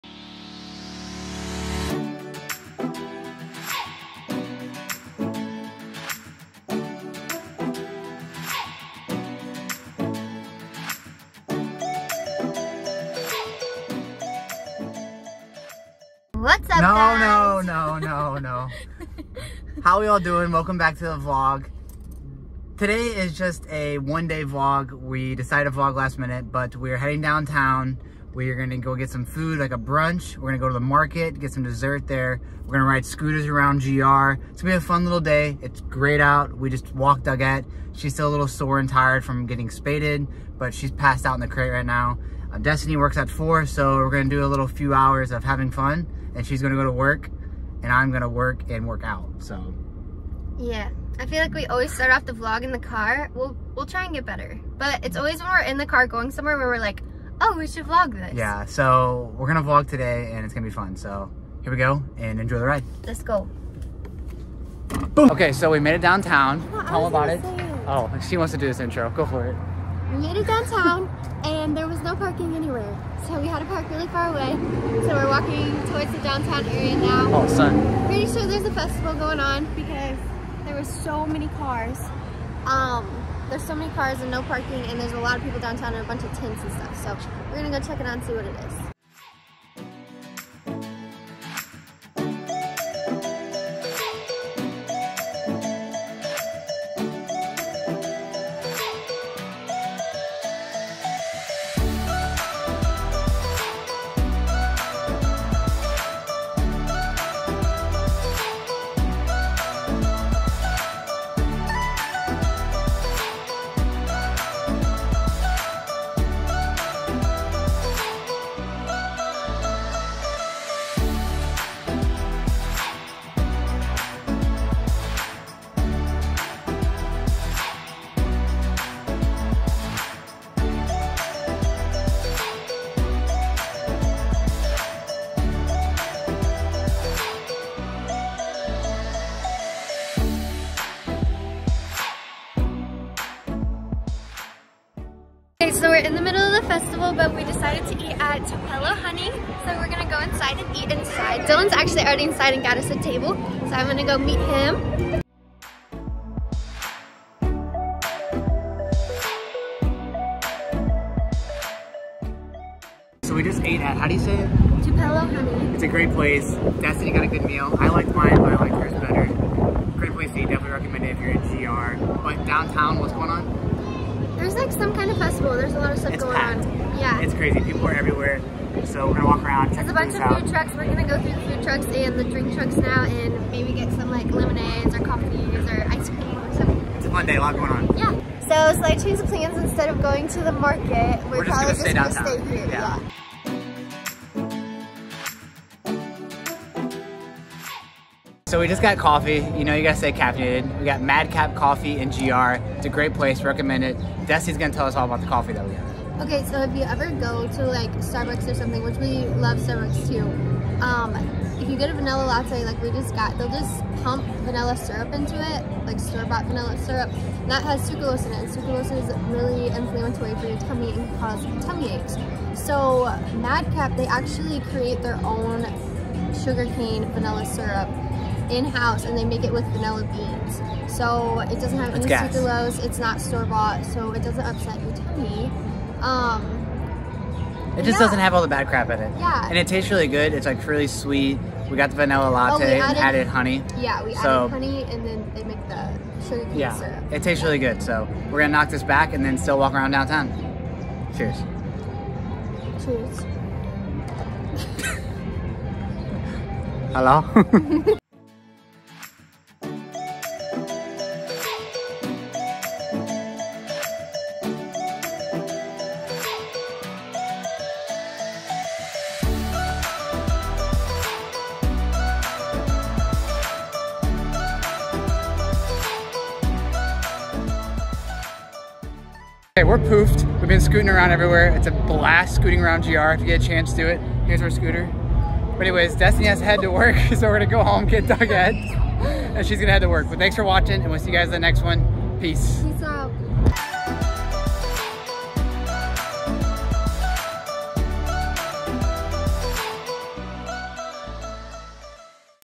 Exploring Grand Rapids Michigan!! Scooter rides, Coffee, Great food, + more :)
We decided to go to downtown Grand Rapids last minute and enjoy a lunch date in the city!

We ran into a packed city because of a music festival going on! We checked everything out, rode scooters everywhere, got our favorite coffee, and ate some amazing food!

Madcap Coffee Company: 98 Monroe Center St NW, Grand Rapids, MI 49503

Tupelo Honey: 140 Ottawa Ave NW STE 100, Grand Rapids, MI 49503